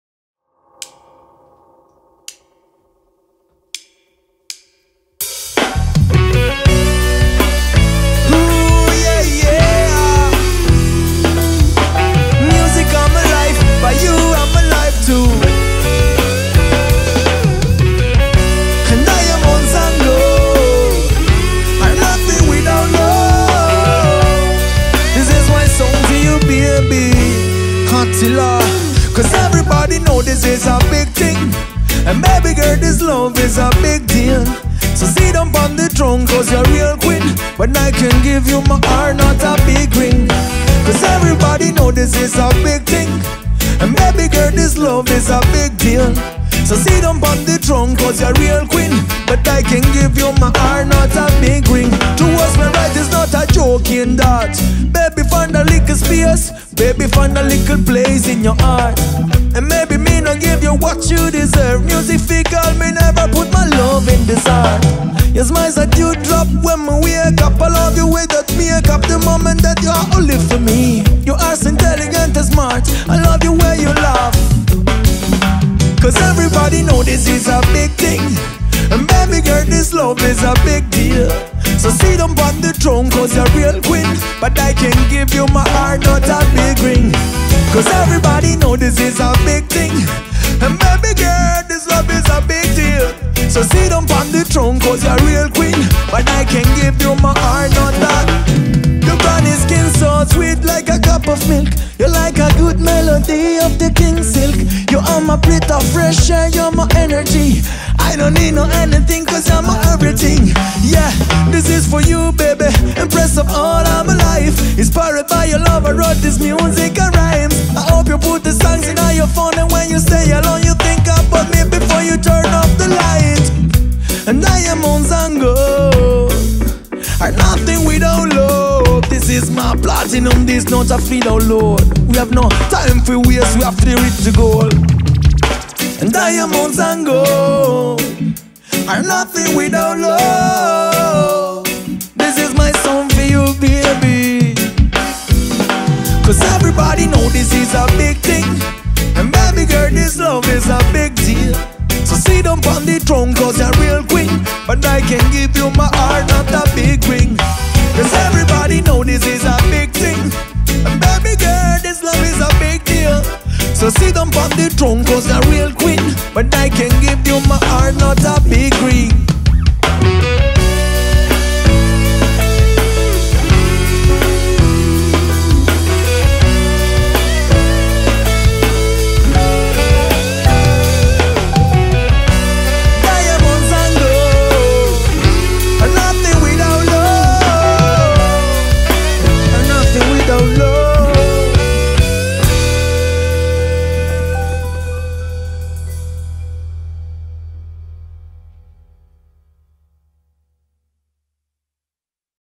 Real Queen Attila & Eazy Skankers
Live in studio with Eazy Skankers

Tastiere: Gianni Pastorino
Basso: Andrea Bottaro
Guit: Simone Amodeo
Batteria: Massimo Minato
Registrato @Bombastik Studio ...Imperia